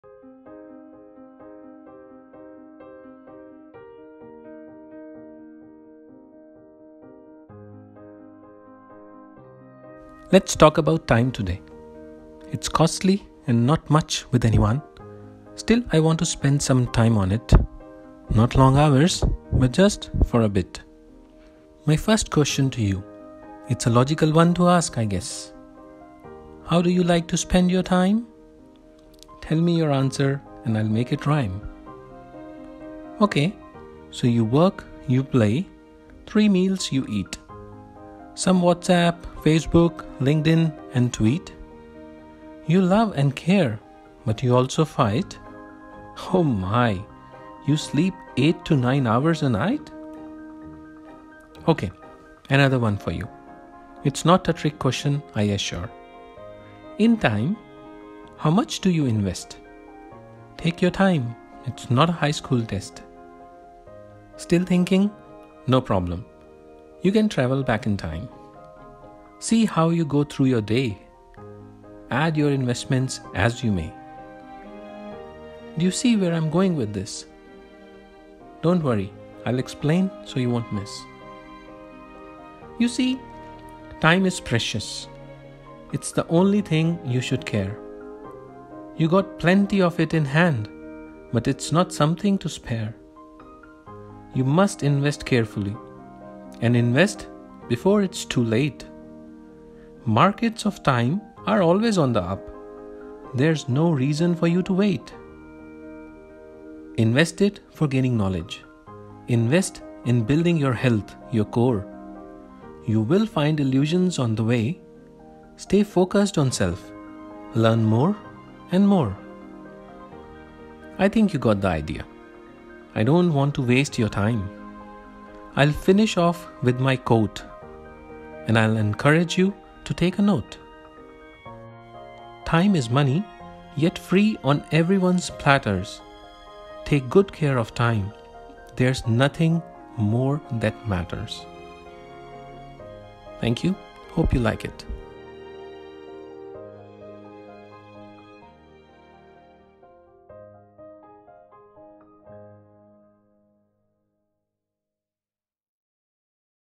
Do you have TIME??? It's ticking away ... Tick Tock ... Tick Tock ... Tick Tock
A quote from Warren Buffett said 'The rich invest in time, the poor invest in money.'

What do you invest in, 'TIME' or 'MONEY'?

There are a lot of quotes you will find on google but ultimately, it is you who has to decide how to make use of your time. Once the time is gone, it will not return back. So make careful use of time, all that you have it in hand.

Cheers & Inner Peace!!!